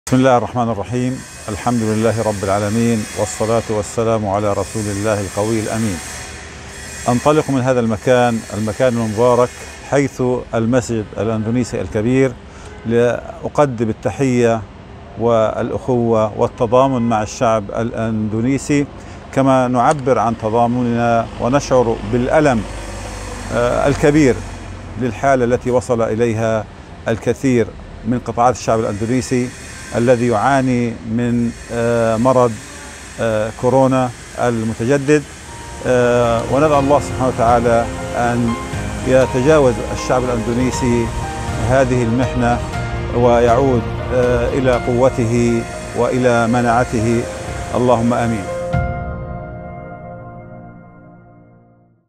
Doa untuk Corona Indonesia - Kasih Palestina
"Semoga bangsa Indonesia kembali kuat dan sehat"

Sahabat Kasih Palestina, pandemi yang sedang melanda Indonesia nyatanya juga menjadi perhatian saudara-saudara kita di Palestina.

Di hadapan Masjid Istiqlal Indonesia di Gaza, penduduk Palestina menyampaikan dukungan dan doa untuk Bangsa Indonesia.

Penduduk Palestina turut berempati atas banyak sekali orang Indonesia yang terjangkit virus corona jenis baru.

Mereka pun berharap semoga Allah SWT memberikan kekebalan dan kekuatan untuk dapat segera melewati ujian ini.

Dukungan serta untaian doa dari Palestina benar-benar memberikan arti yang besar bagi Bangsa Indonesia yang masih berjuang hingga saat ini.

Terima Kasih ya Palestina. Semoga saling mendoakan dalam kebaikan akan semakin mempererat hubungan persaudaraan yang telah kita miliki.